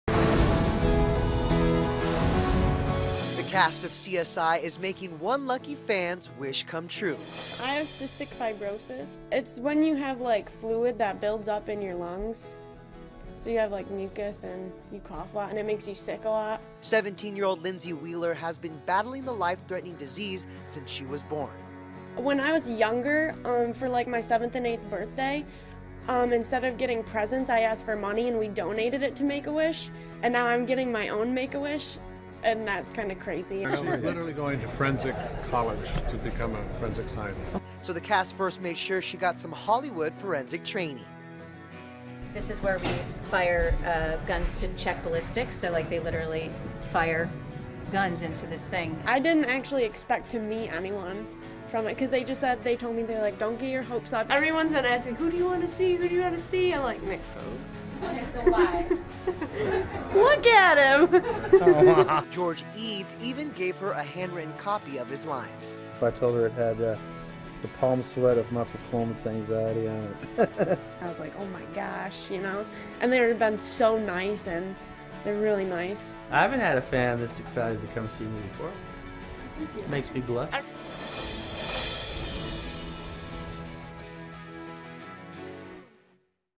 'CSI' Cast Makes a Fan's Wish Come True
One of CSI: Crime Scene Investigation's biggest fans, 17-year-old Lindsey Wheeler, had one of her wishes come true when she met her favorite cast members.